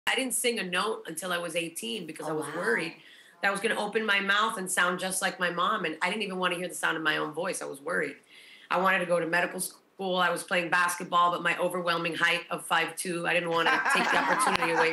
Emily Estefan: Being ‘Outed’ for Her Sexuality, Colorism in the Latino Community, & ‘Red Table Talk’
Singer/songwriter Emily Estefan is the daughter of superstar singers Emilio & Gloria Estefan, and while growing up under a microscope can be challenging, Emily has found her footing navigating the world of celebrity. She opens up about the heartbreaking moment she was outed about her sexuality, colorism in the Latino community, what to expect from ‘Red Table Talk: The Estefans,’ and more!